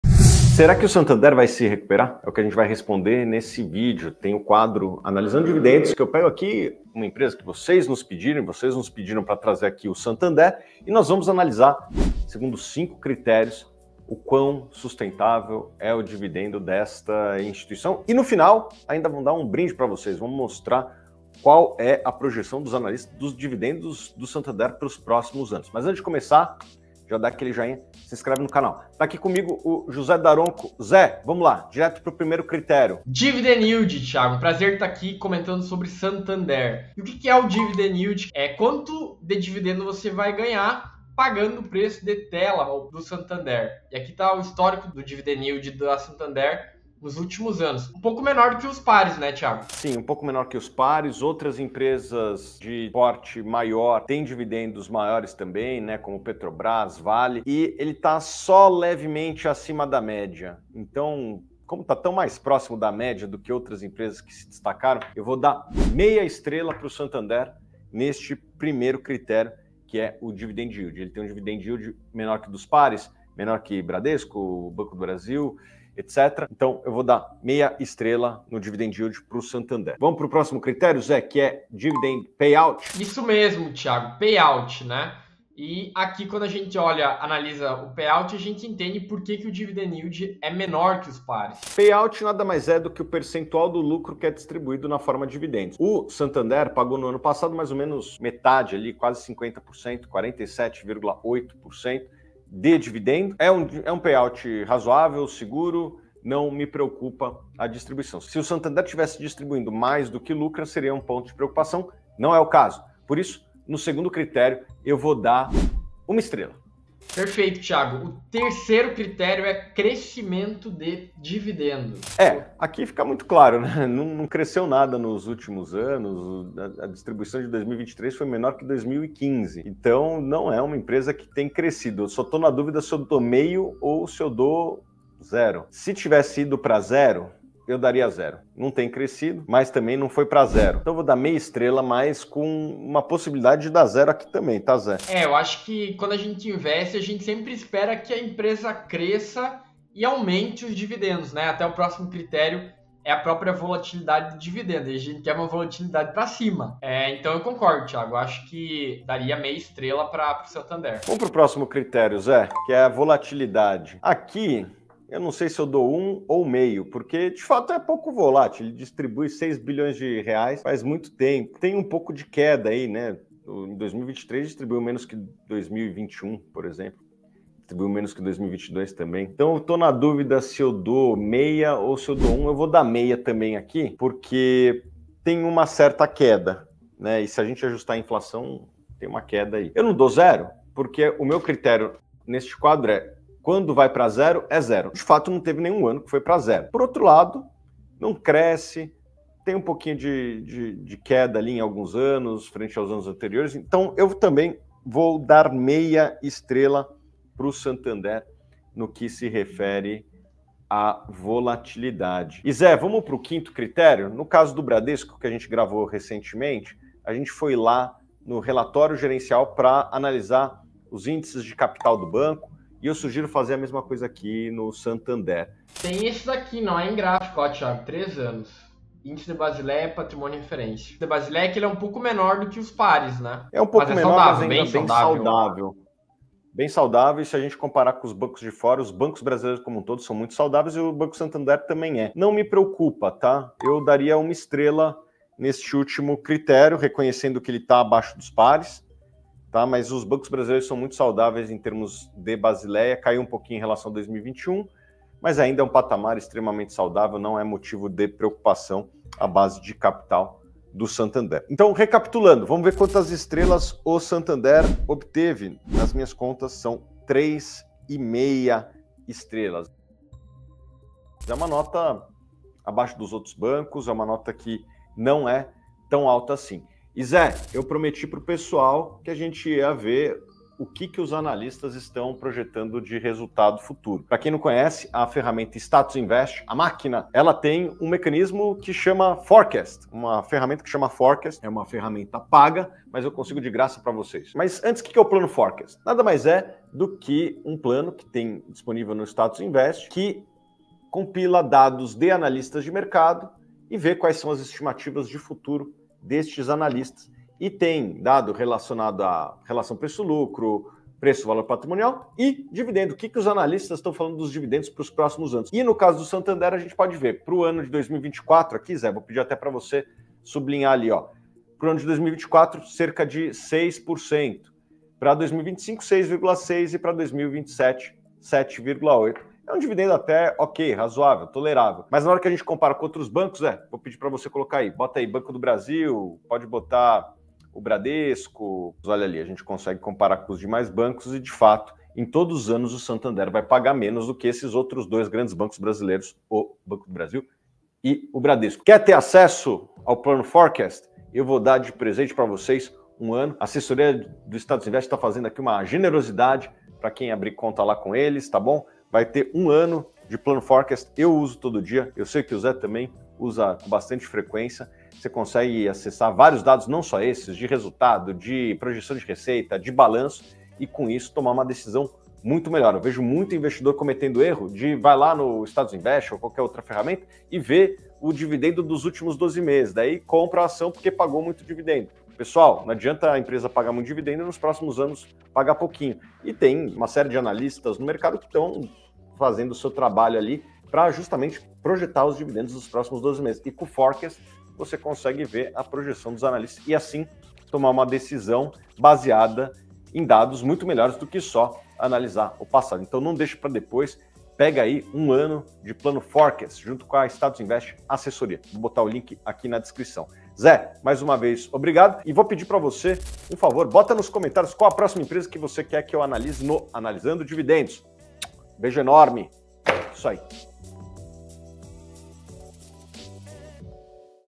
SANB11: Dividendos do Santander de ZERO a CINCO! VAI SE RECUPERAR?
Torne-se cliente do Status Invest Assessoria, a Assessoria de Investimentos que mais cresce no Brasil, e GANHE 1 ANO DO PLANO FORECAST ➡️

SANB11: Nota dos Dividendos do Santander de 0 a 5! VAI SE RECUPERAR?

Será que o Santander vai se recuperar? No vídeo de hoje, vamos responder a essa pergunta com uma análise detalhada dos dividendos do Santander usando cinco critérios essenciais.

Dividendo sustentável ou armadilha para investidores?
Vamos analisar o dividend yield do Santander e ver como ele se compara com seus principais concorrentes, como Bradesco e Banco do Brasil. Vamos mostrar também as projeções de dividendos do Santander para os próximos anos. Será que vale a pena investir?

Além disso, você vai ver uma comparação detalhada do Santander com outros bancos em termos de:

 - Dividend yield
 - Payout ratio
 - Crescimento dos dividendos
 - Volatilidade dos dividendos
 - Índices de capital

E para facilitar sua vida, o vídeo está dividido em partes importantes:

1. Análise do Dividend Yield e Payout Ratio
2. Crescimento e Volatilidade dos Dividendos
3. Projeções Futuras e Comparação com Concorrentes

Quer um conselho? Assista ao vídeo até o final. Tem uma surpresa especial para quem nos acompanha.

Conteúdo do vídeo:
00:00 Introdução
00:33 Dividend Yield
01:22 Payout Ratio
02:01 Crescimento de Dividendos
02:41 Volatilidade de Dividendos
03:45 Índices de Capital
05:12 Plano Forecast e Conclusão

Santander: Vai Se Recuperar? Analisamos os Dividendos!